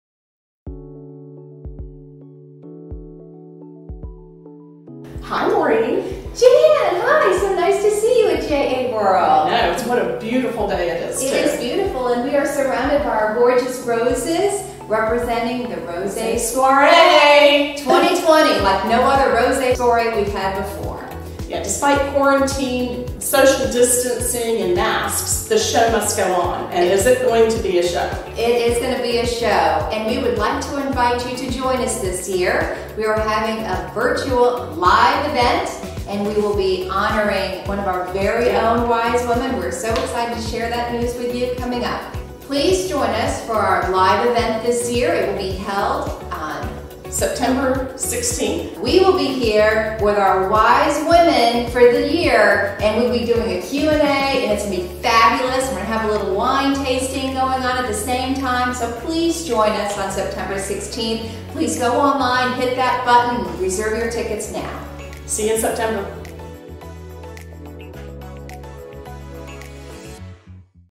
Junior Achievement of South Florida 2020 Rosé Soirée Promo 1
Now an exciting virtual event on September 16, 2020 - join co-chairs Maureen Canada, President & CEO of Lighthouse Point Marina and Nauti Dawg Marina Cafe, and Jan Rover Nouss, Principal of TN Partners, Inc. and the JA Circle of Wise Women to sip and swirl Rosé and support Junior Achievement of South Florida.

JA’s Rosé Soirée celebrates the amazing women of our “Circle” and this year will recognize the 2020 Woman of Wisdom (WOW) honoree, Susan Renneisen, Vice President of Community Affairs & Special Events at Seminole Hard Rock Hotel & Casino. The event also includes the pinning the newest 2019-20 members of JA’s Circle of Wise Women and the awarding of three very deserving female students with the CWW Girls Scholarships.

Proceeds from Rosé Soirée will support the young girls of JA programs, including college/career certifications, scholarships, summer camp, JA Career Bound and JA Fellows programs. Remember to wear pink!